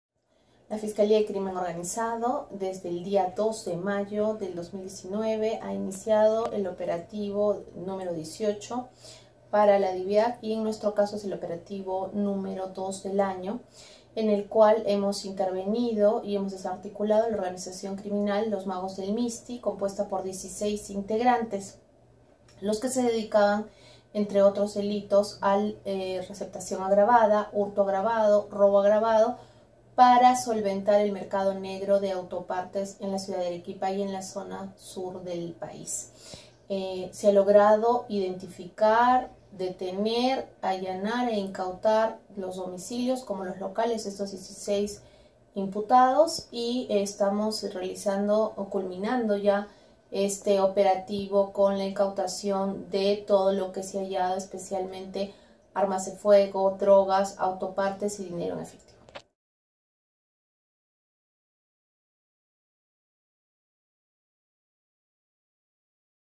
Detienen a 16 involucrados en la presunta organización criminal "Los Magos del Misti"
El Ministerio PúblicoPolicía Nacional del Perú desarticularon la organización criminal 'Los Magos del Misti', que se dedicaría al robo de autopartes en Arequipa. Los 16 detenidos serán investigados por los presuntos delitos de receptación, robo, hurto agravado y organización criminal.